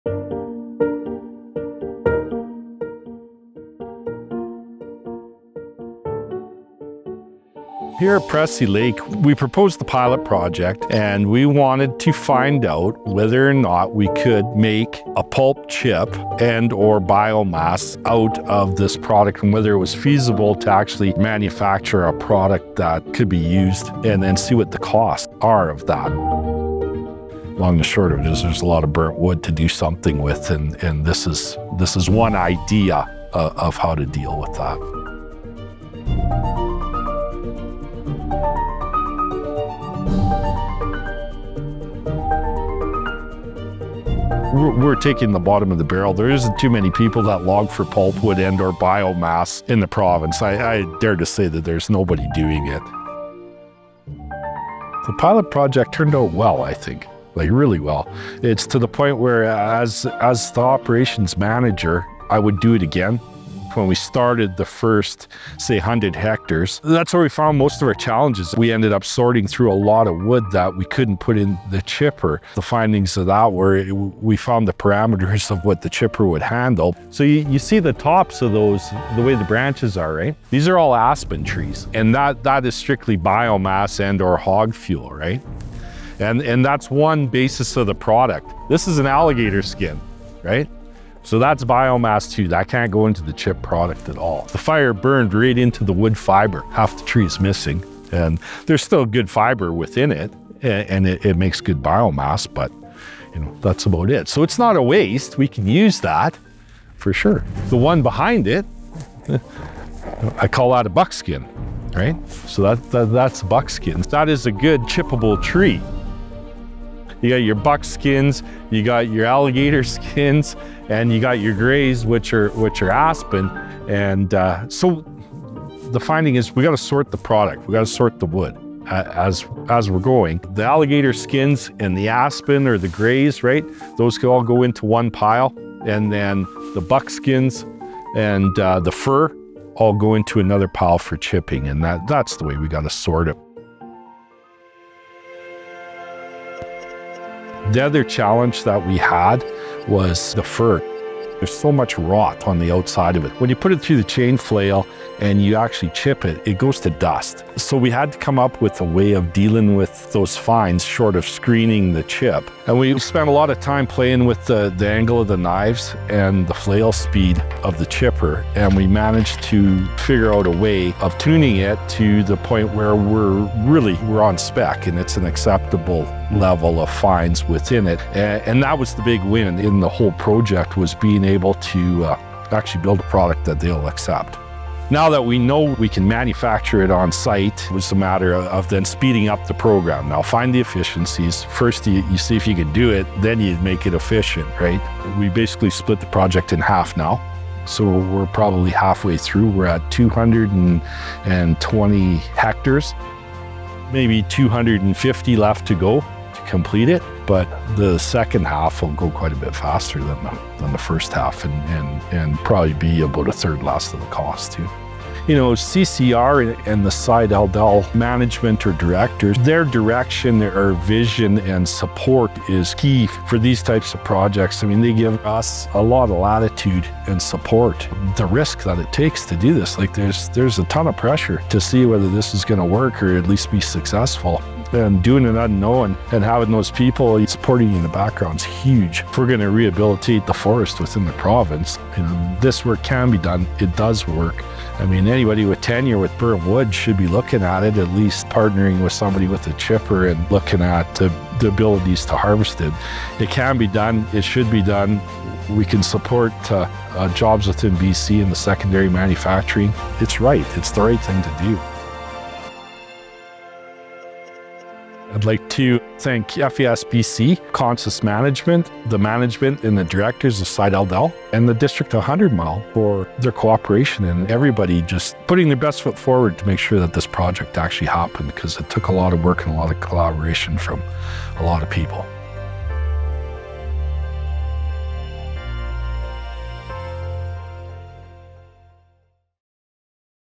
Central Chilcotin Rehabilitation Ltd.'s Forestry Pilot Project at Pressy Lake: "What we Learned"
In light of the ongoing challenges facing the forest industry, including mill closures, timber supply shortages and unprecedented wildfire seasons, Central Chilcotin Rehabilitation Ltd. (CCR) recognized the necessity of creating new opportunities within the sector.

New initiatives, such as a pilot project to rehabilitate the 2017 Elephant Hill wildfire-affected area at Pressy Lake, with support from the Forest Enhancement Society of BC funding, are an attempt to bring balance to the sector.

Extracting 7-year-old burned fibre poses significant challenges owing to the brittleness of standing dead trees and complexities in management.

In this stage, the CCR team hauled the biomass products to their end users with help from Forest Enhancement Society of BC funding.

According to Daniel Persson, Registered Professional Forester and CCR’s Forestry Superintendent estimates it will see up to 1,000 truckloads of biomass created from this project if it is successful. This is just the starting point of this project. The long-term objective is to have a fully regenerated forest left for future generations. This includes several phases and will take at least 3 years to complete, and that is when we mark this project as complete.

“Every phase depends on the one before it, and our overall timeline from start to finish is flexible with our target to complete it over 3 years,” noted Persson. “We look forward to what we will learn and how we can share this knowledge with others in the industry.”

This is part two of the project. Part three will be replanting the land - video to come soon!